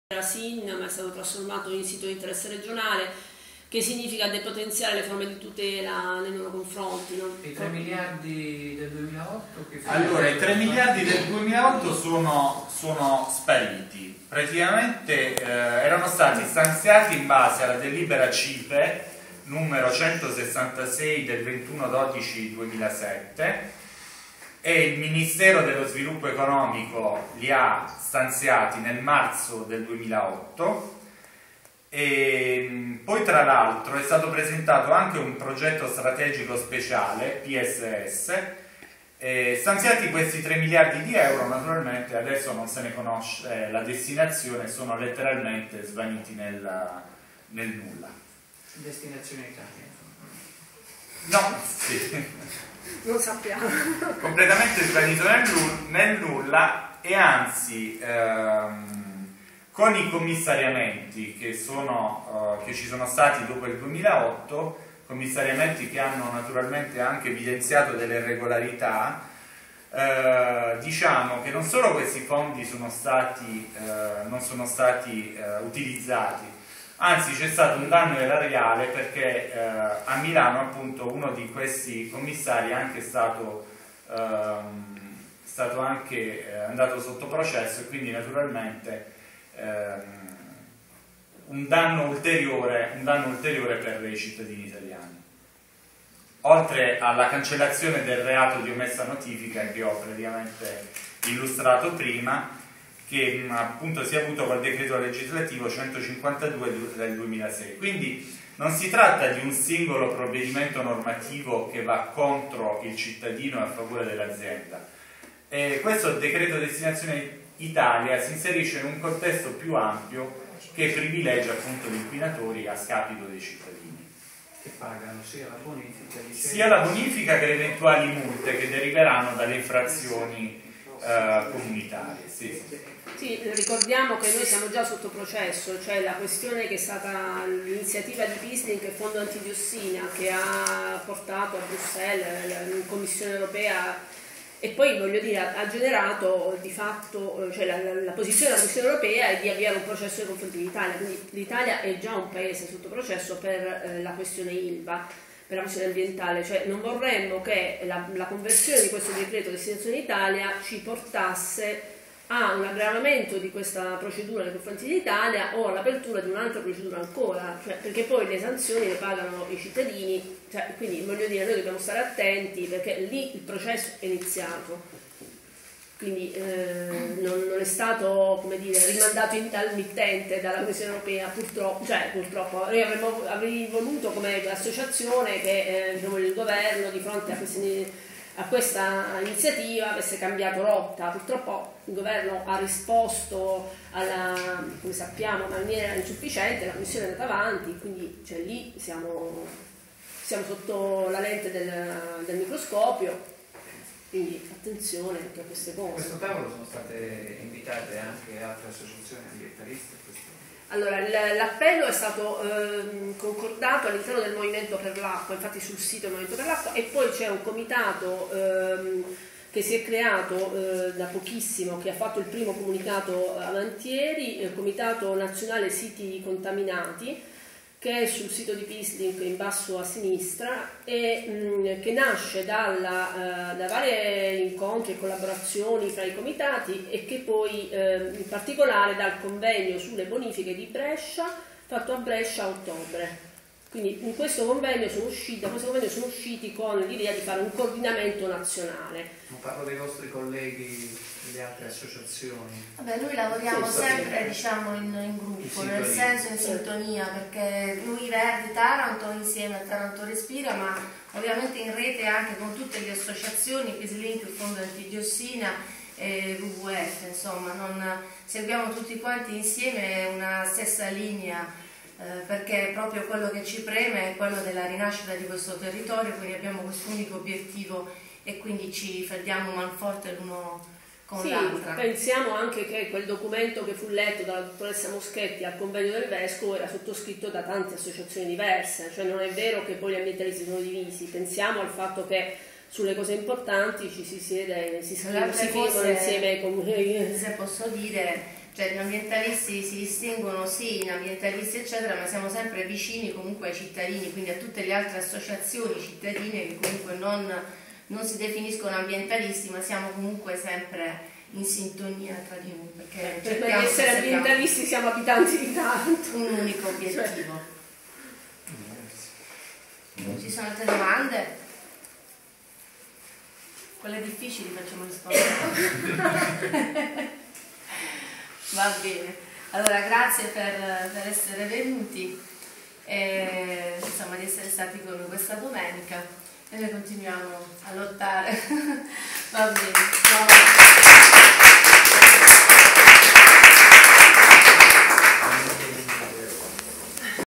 CAM01801 Conferenza stampa dei Verdi Taranto "Condono tombale agli inquinatori!" 9/02/2014
Domenica 9 febbraio alle ore 11:00 presso la sede di Taranto Respira in via Oberdan 111 è convocata una conferenza stampa con Ada Le Noci e Luca Piccione, coportavoci dei Verdi Taranto
sul disegno di legge di conversione del D.L. 145/2013, c.d. "Destinazione Italia" , che reca misure incidenti su numerosi settori e il cui filo conduttore dovrebbe essere, secondo quanto esplicitato nel preambolo del decreto stesso, il rilancio della competitività delle imprese.
Si ringrazia il movimento civico Tarantorespira per l'ospitalità gentilmente concessa.
Associazione cittadina Verdi Taranto
Ada Le Noci
Luca Piccione
Coportavoci dei Verdi Taranto